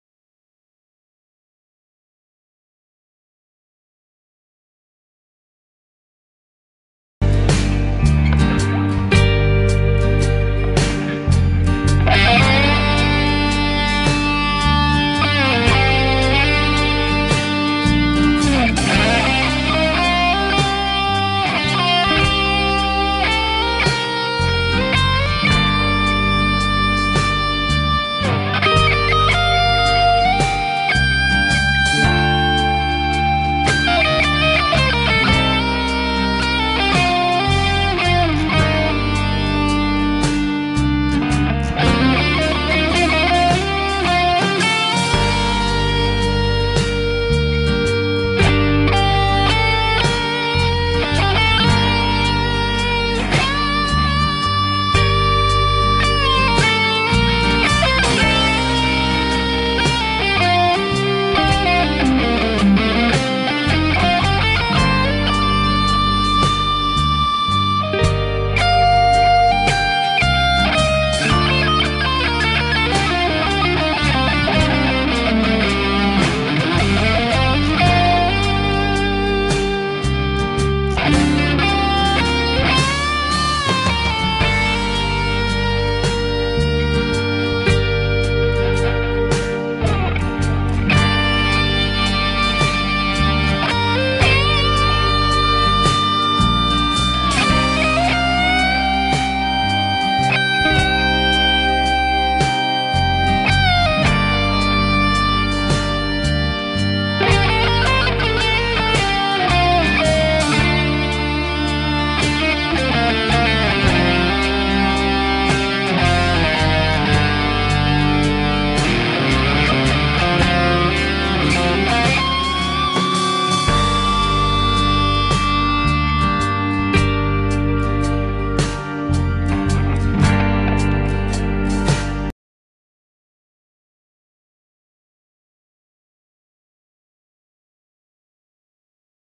Greco WS 40 Wild Scamper PPK 2012 & Kz TrebleBooster+TUBESCREAMER TS808+Orenge DarkTerror 004
Greco WS-40 Wild Scamper PPK 2012
   FGI-H(R) Pickup
Kz GuiterWorks Kz TrebleBooster
TUBESCREAMER Overdrive Pro TS808
Boss GE-10
Maxon DD-1000
Orenge DarkTerror
Marshall JCM9001960A